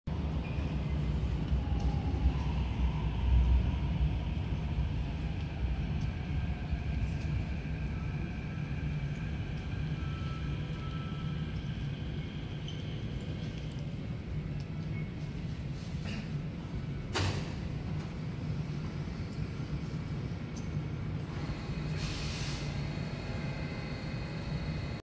SMRT Kawasaki Heavy Industries CRRC Qingdao Sifang CT251 2001 arriving Woodlands North (14)
Date and time recorded: 18 November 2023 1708

Train: Kawasaki Heavy Industries & CRRC Qingdao Sifang CT251
Trainset: 2001
Year entered service: 2020
Line: Thomson-East Coast Line
Destination: (14) Gardens by the Bay